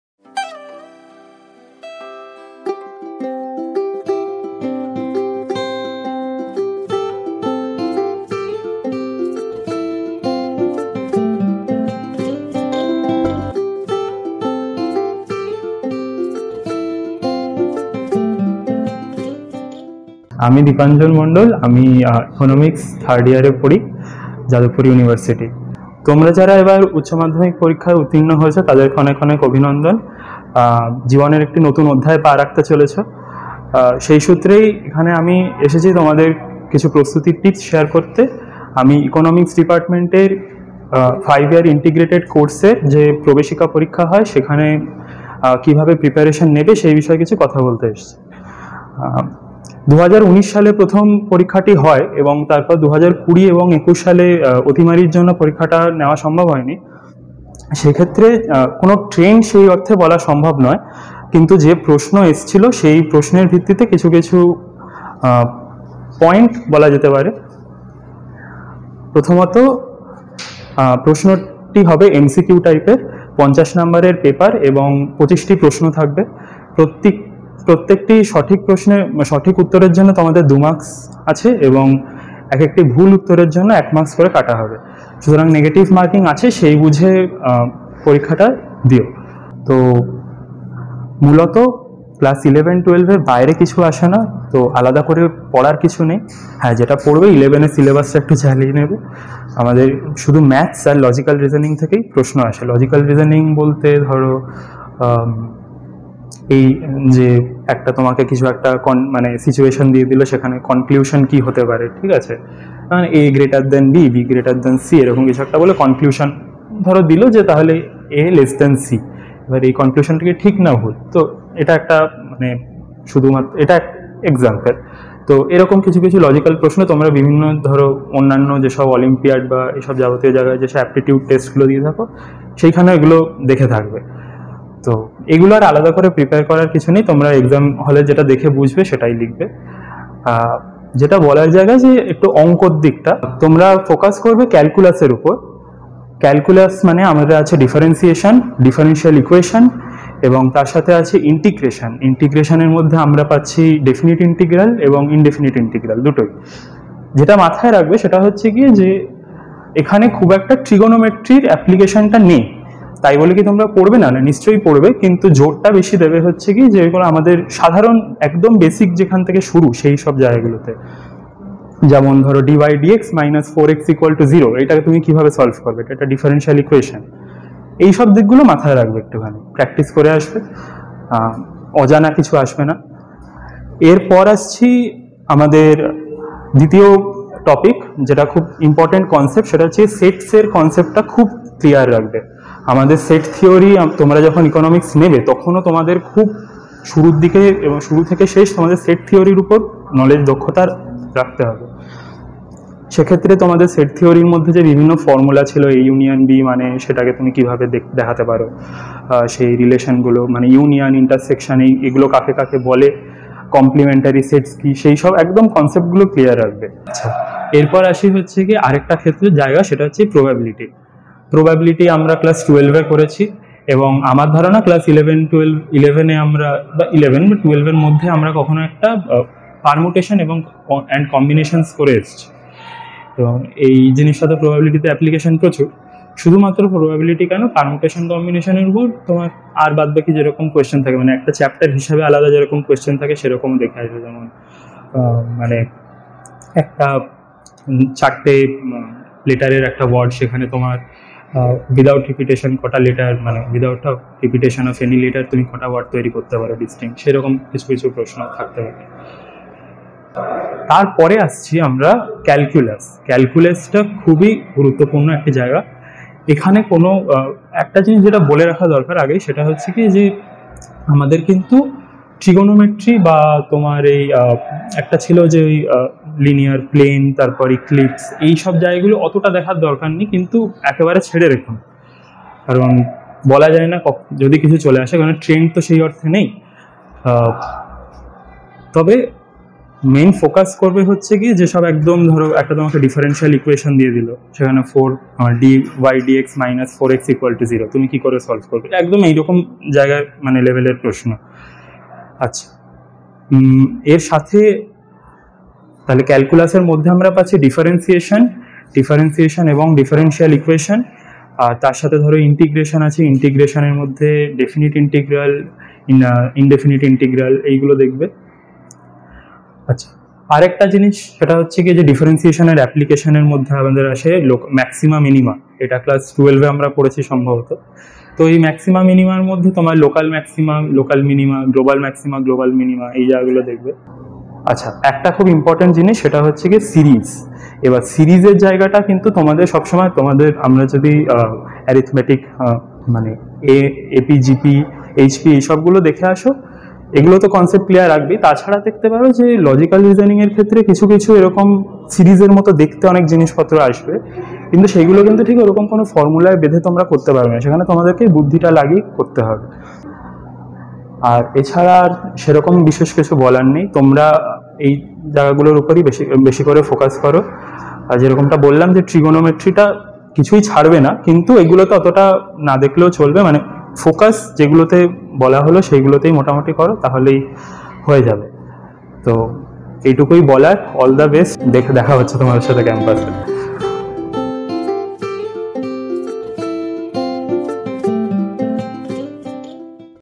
Tips for Admission test of B.A. in the Department of Economics, Jadavpur University.
"প্রস্তুতি tips" by Students' Federation of India, Jadavpur University Local Committee for the students who are appearing the admission test for B.A. Economics. More episodes with other courses and departments are coming soon...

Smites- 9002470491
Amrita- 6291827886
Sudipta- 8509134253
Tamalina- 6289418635